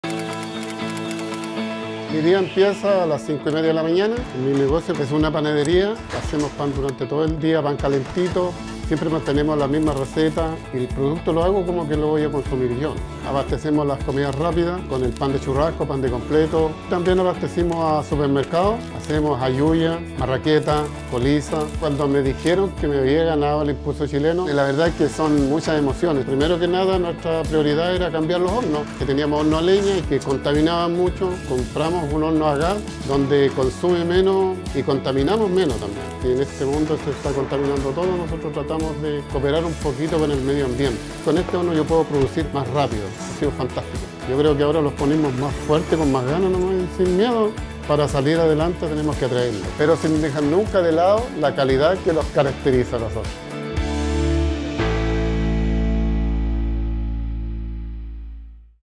"Panadería Andrés" de Rodolfo Arcos
De hornos a leña a hornos a gas. Esa fue una de las grandes inversiones que hizo Rodolfo Arcos, ganador de la tercera versión de ImpulsoChileno, en su panadería gracias al premio. “Produzco más y consumimos y contaminamos menos. Con esto tratamos de cooperar un poquito con el medio ambiente”, dice el emprendedor de Toltén.